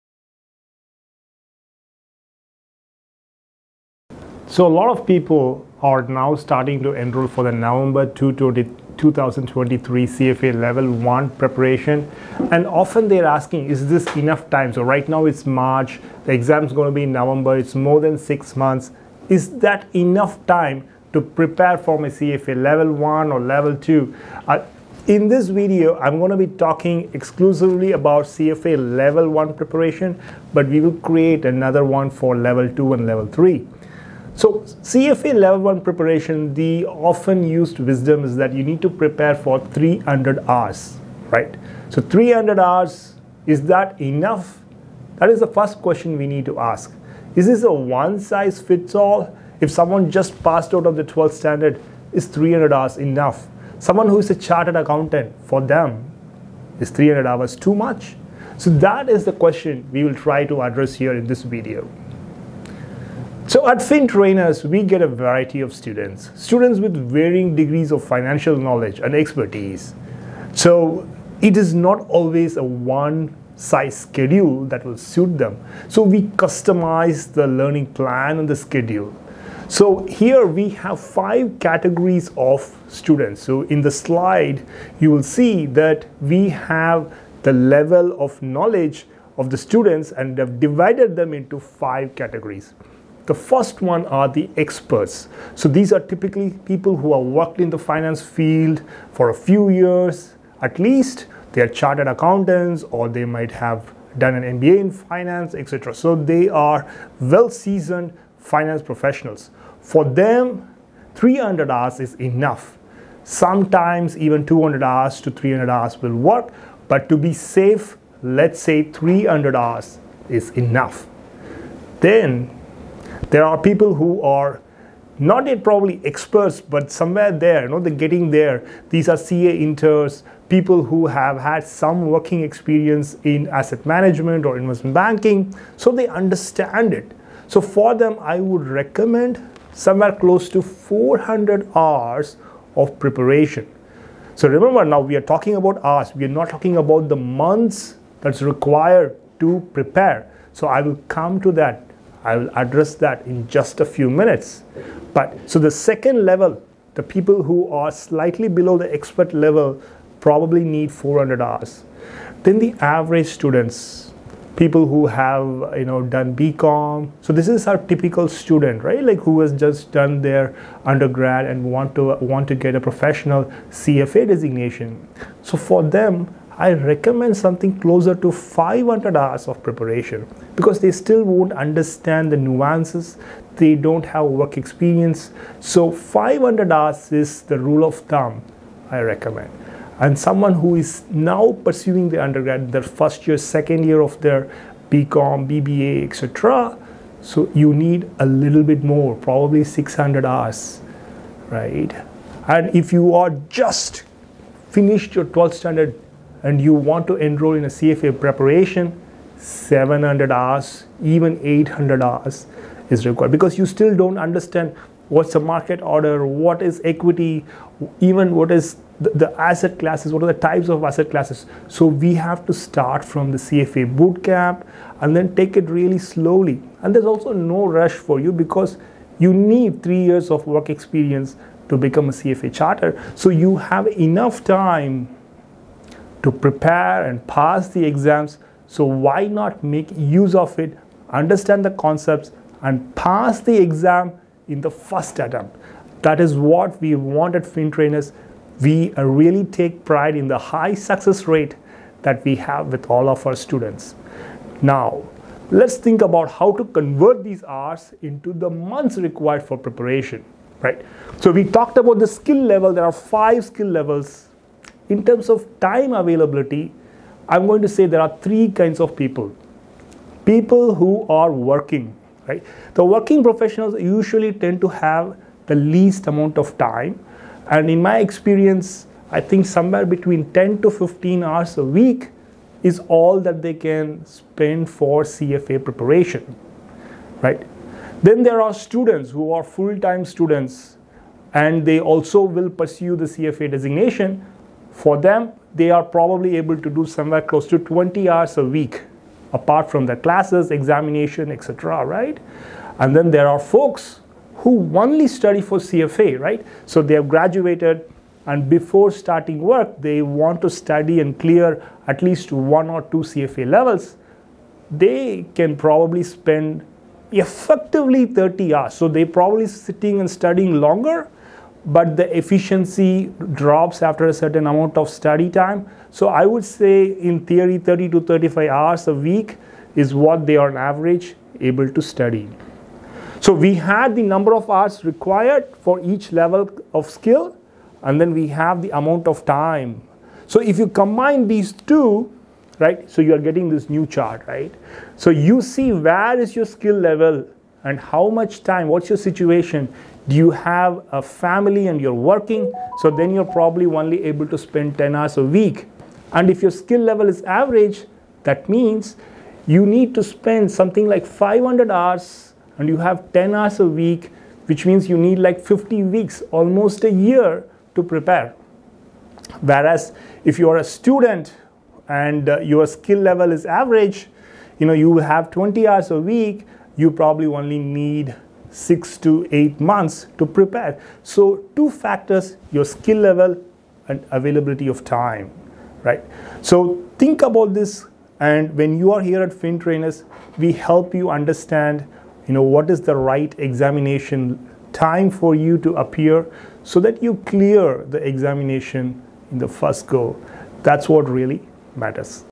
CFA Level 1- Prep time | How much time do you need?
What is the right amount of time to allocate for CFA Level 1 Prep?

Here our chief Trainer looks to answer this question that most student ponder about

CFA Charterholder is a coveted designation and it is an arduous task. So, providing the right amount of time for preparation is critical